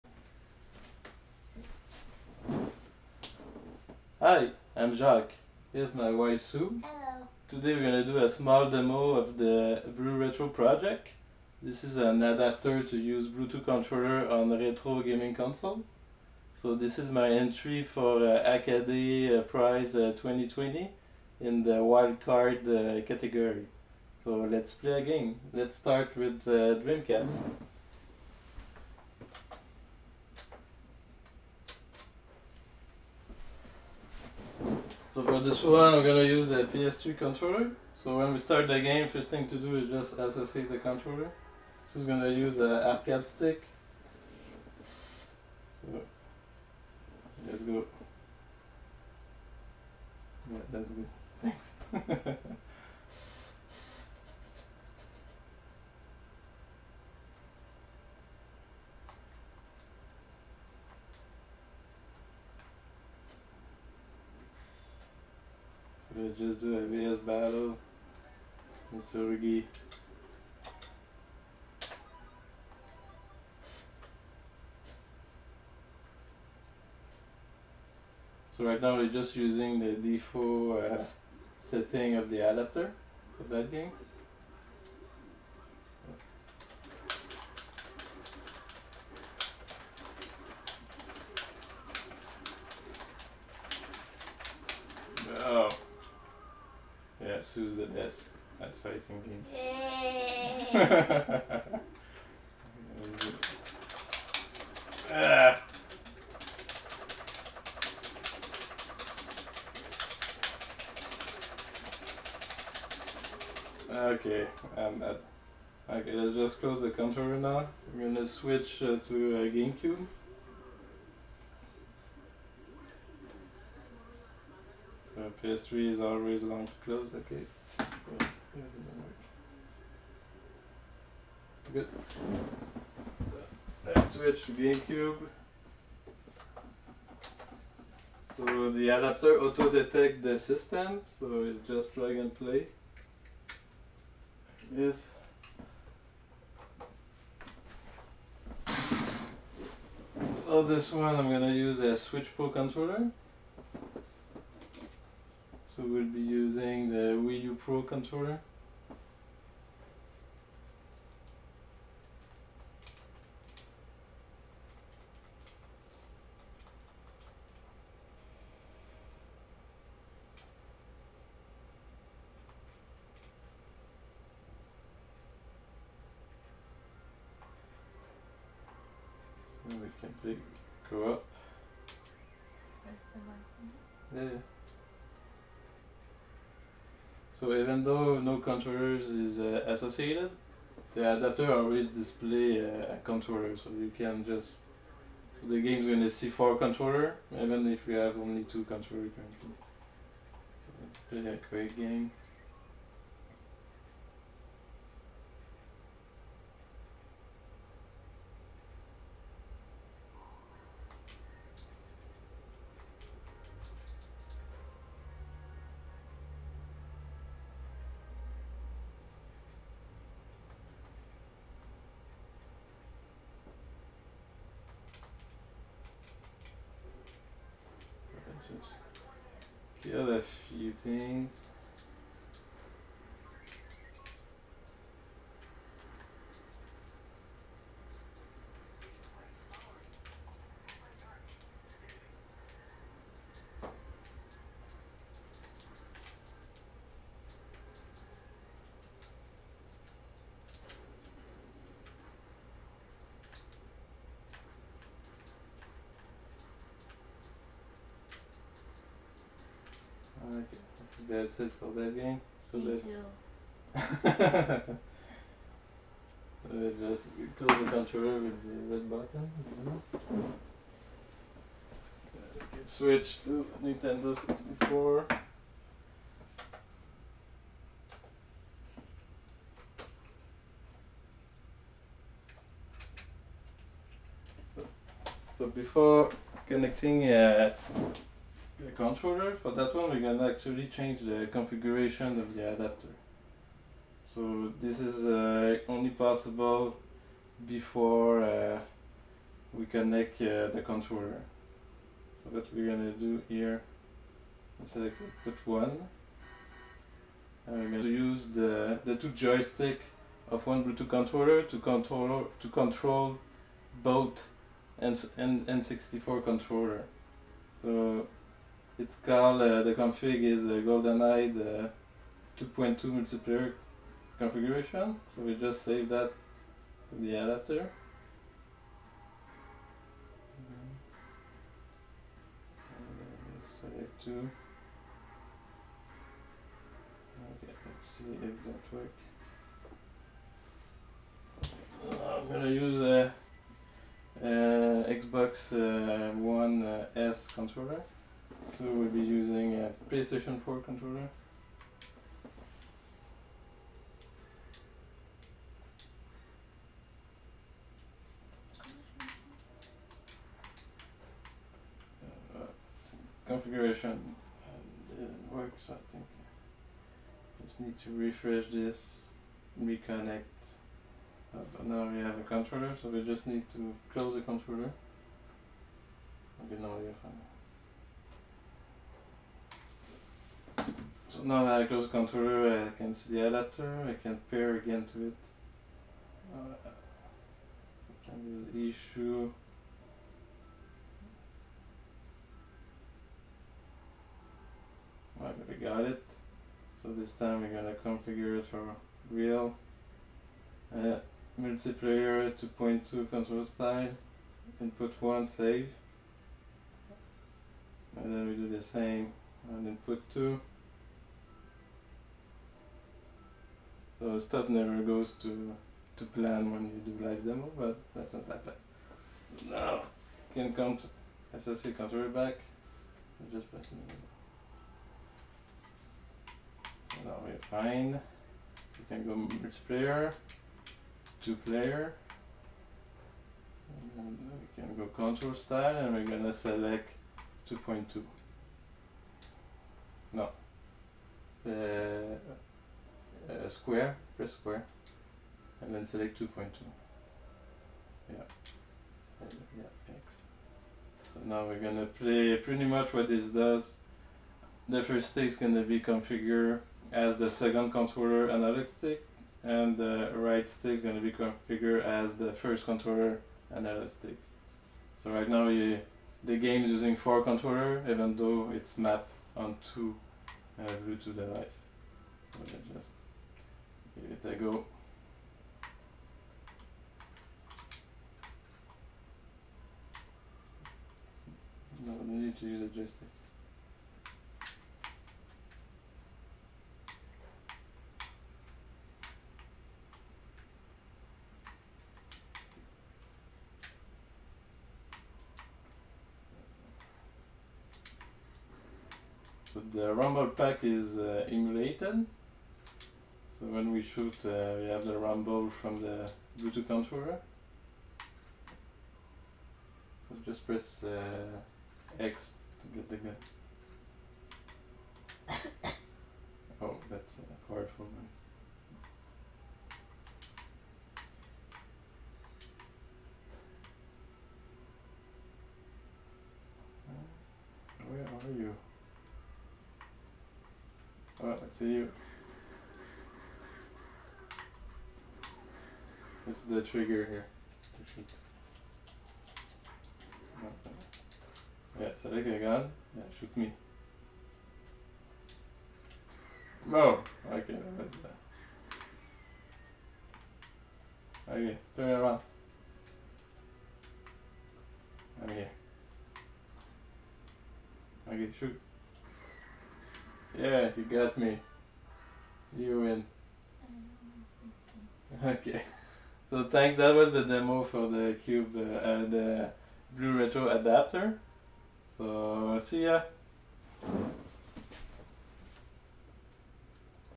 BlueRetro - Multiplayer Bluetooth controllers adapter for retro video game consoles WIP ESP32
BlueRetro is a multiplayer Bluetooth controllers adapter for various retro game consoles. It is open source hardware & software. The software is build for the ESP32 chip. All processing for Bluetooth and HID input decoding is done on a single core which make it easy for other projects to use the Bluetooth stack within their own projet as long the application is running on the other core. This is a spiritual successor in some way to my project fork. This is my wildcard entry to the 2020HACKADAYPRIZE